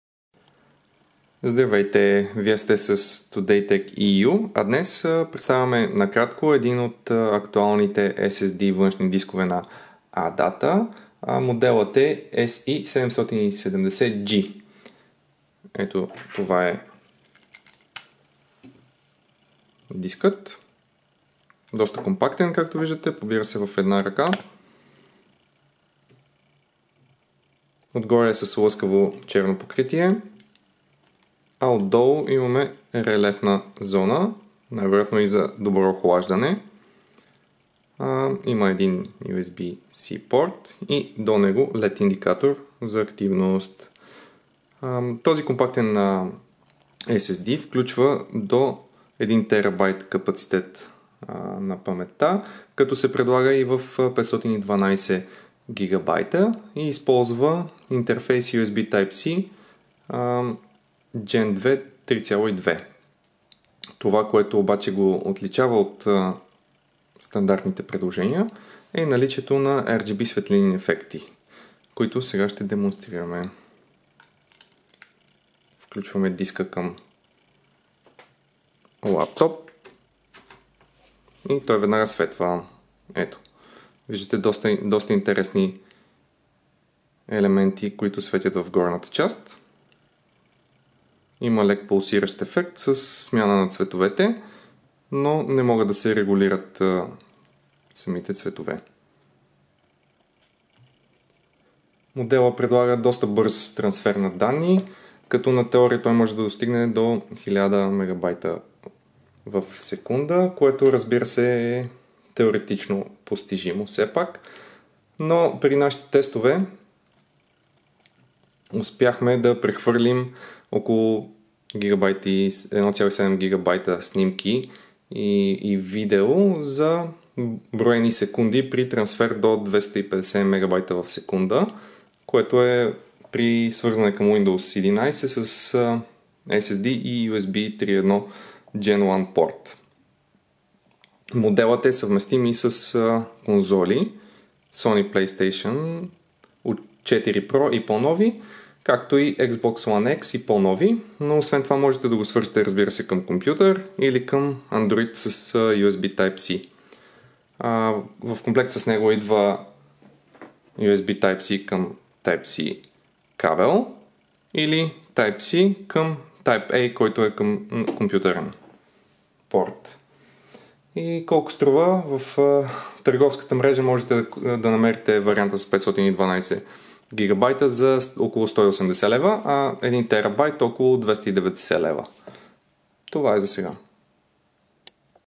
ADATA SE770G – външен SSD диск с RGB ефекти
Кратък преглед на портативния външен SSD диск ADATA SE770G RGB